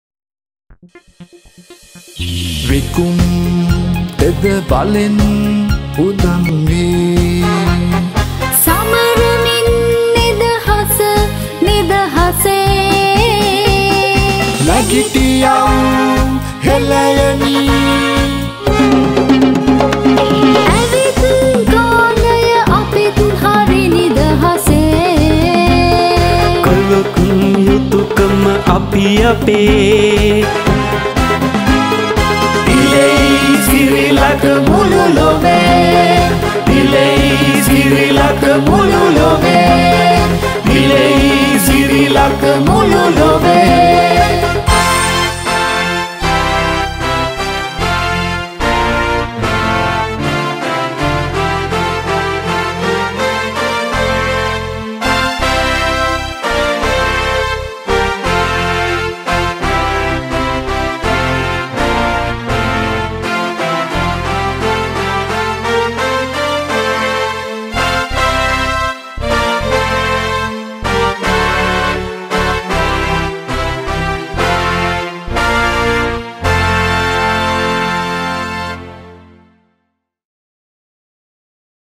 independence day 2020 2 4 Nishi Tennakoon , Suneth Kelum
2020 Independence Day Sri Lanka
Dr. Sameera R Smarakoon ,Nishi Tennakoon
Suneth Kelum
Lyrics   - Dr. Smaeera R Samarakoon
Special Thanks  - Sarath de Alwis